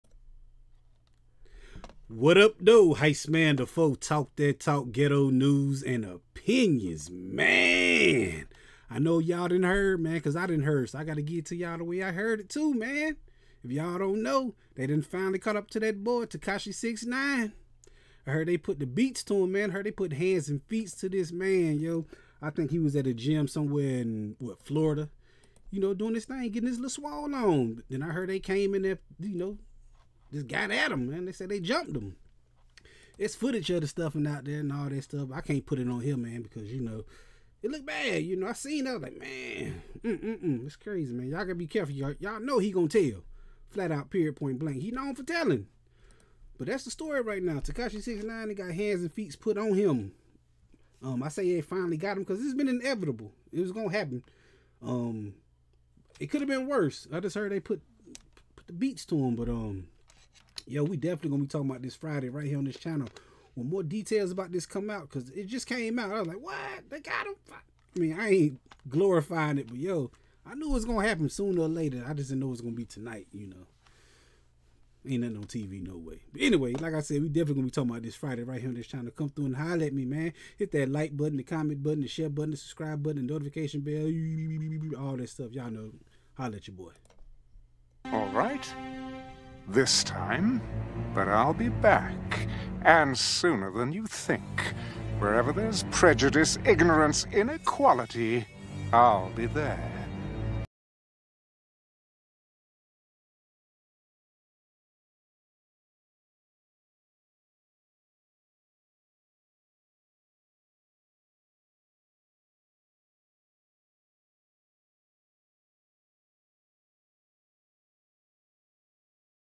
TEKASHI 69 JUMPED at LA FITNESS!!!
Rapper Tekashi 69 was jumped at LA Fitness
Maaaaaaan!!!